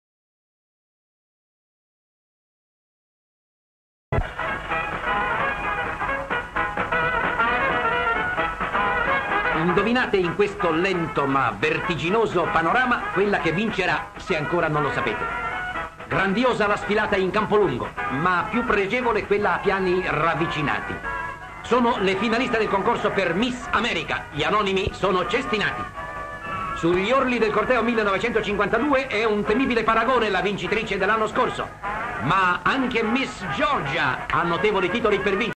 Miss America.
Atlantic City: finali per il concorso di Miss America.
Descrizione sequenze:giovani donne in costume da bagno e fascia trasversale posano per le fotografie ; le ragazze concorrenti su macchine decappottabili sfilano per le strade di Atlantic City ; le ragazze che hanno alle spalle un ventaglio piumato salutano la folla ;